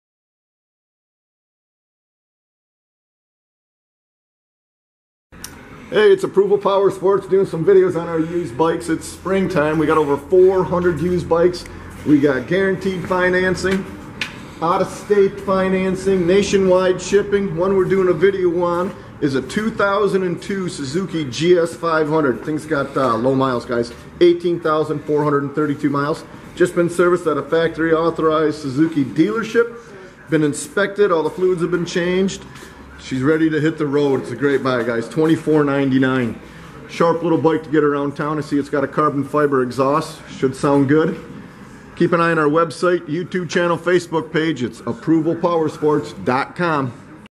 2002 SUZUKI GS500 U5599
USED SUZUKI GS 500 FOR SALE WITH ONLY 18,432 MILES! JUST SERVICED AT A FACTORY AUTHORIZED SUZUKI DEALERSHIP, INSPECTED, ALL FLUIDS CHANGED AND READY TO RIDE! BUY WITH CONFIDENCE! GREAT BIKE FOR THE MONEY!